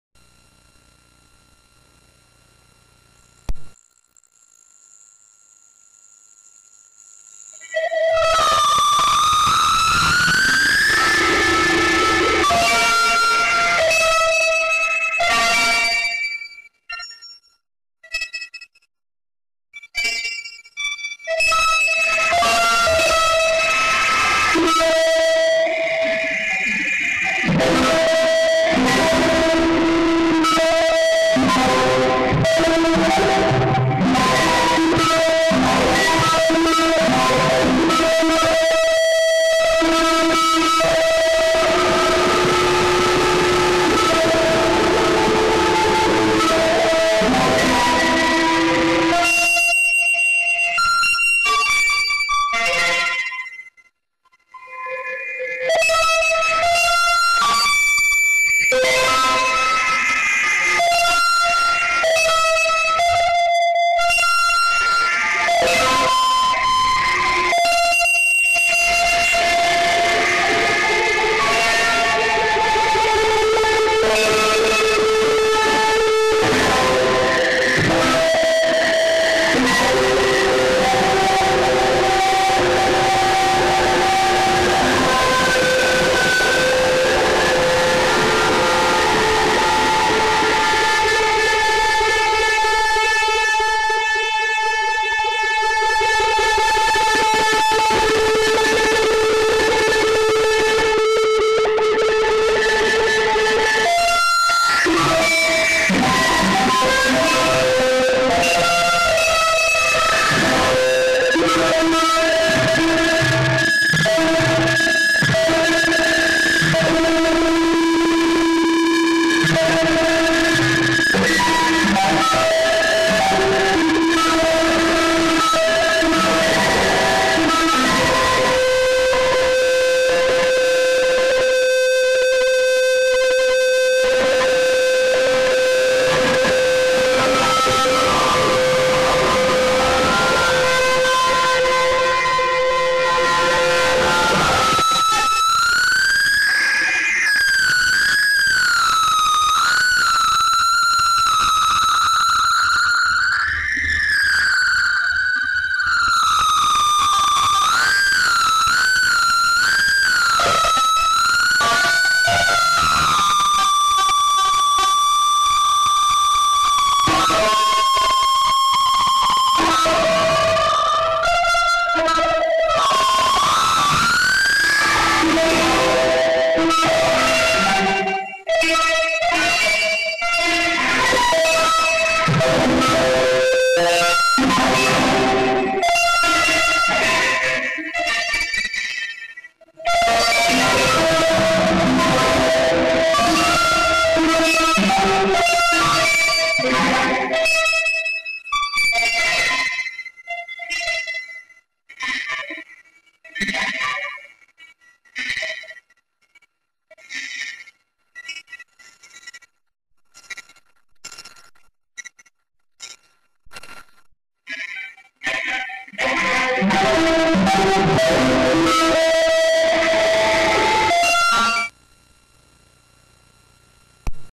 Sound-suit
A movement-controlled audio generation/manipulation system, built from scratch.  Translates gestures and dance into sound.  Built/recorded 2005.

This project is currently shelved while I increase my electromechanical fabrication skillz.  Eventually, I will tackle this project again and make it more rugged and also more expressive.